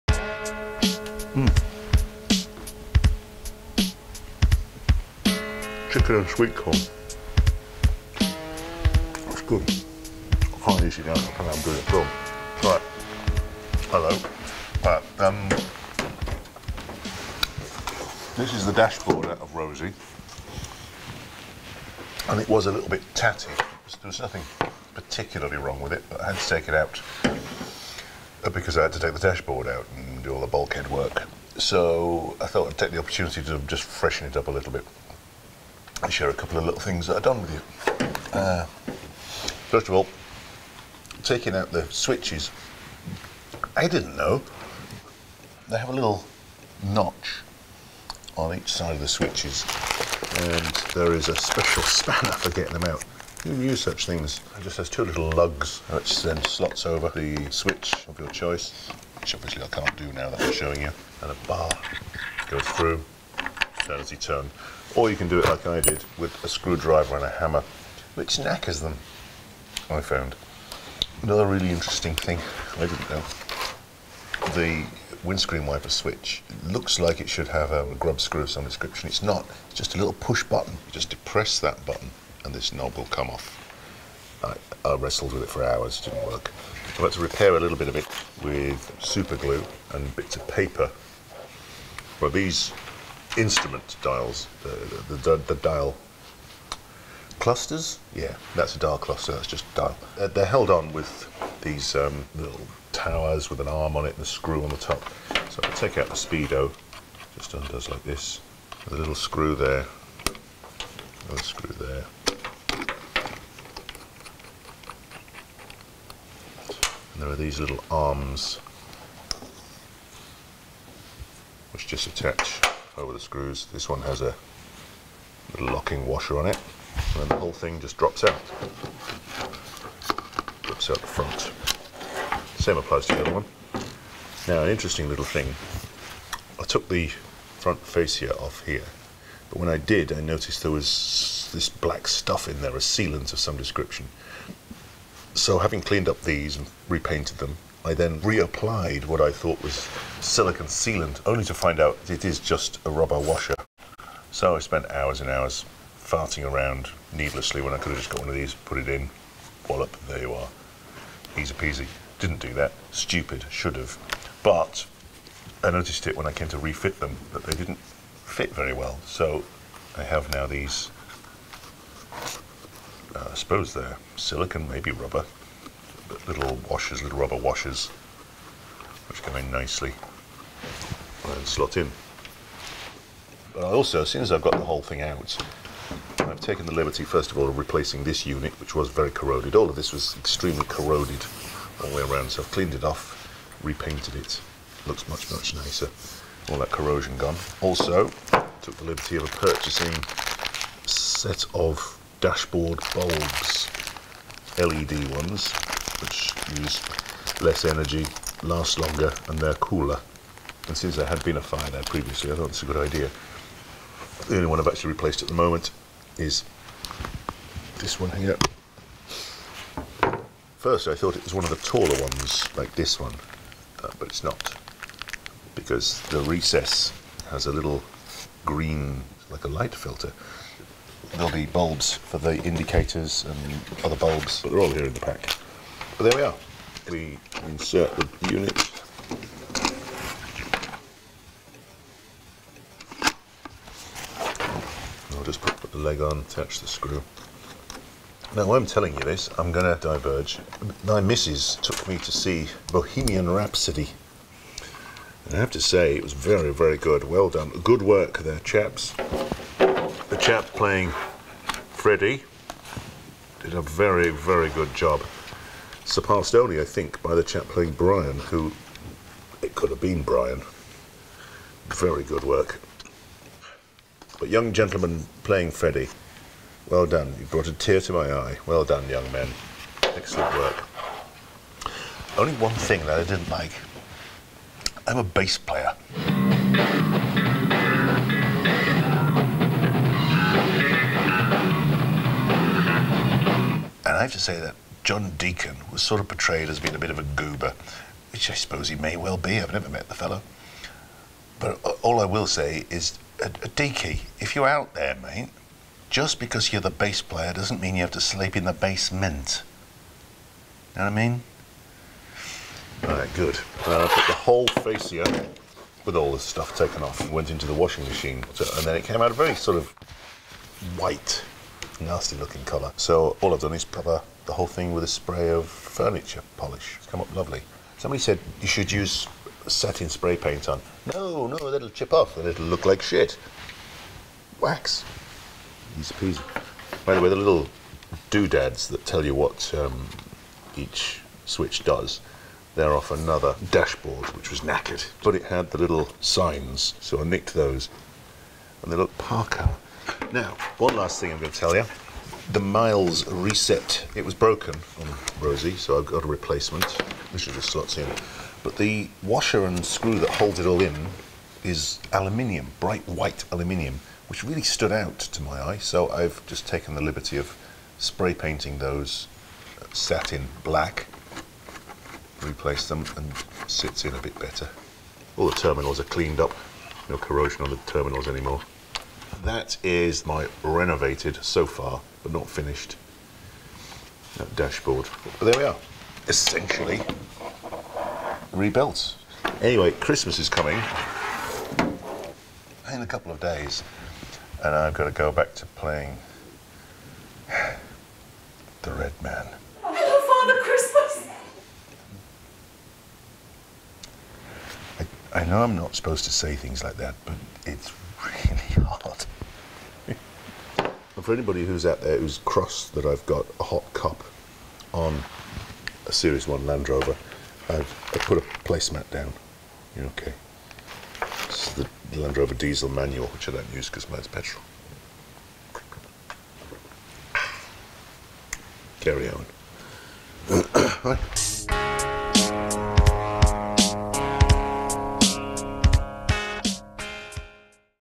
Ep. -02; Land Rover, Series 3 rebuild. "Instrument panel rebuild/restoration/repair"
A few tips about renovation your instrument panel of your dashboard. In 2015, my 1984, series 3, Land Rover was written off on the M40. I am attempting to rebuild/restore/repair her, but I am not a mechanic and have very little knowledge in this field. In fact, I'm like a monkey with a spanner, but if I can do THIS, you can do THAT.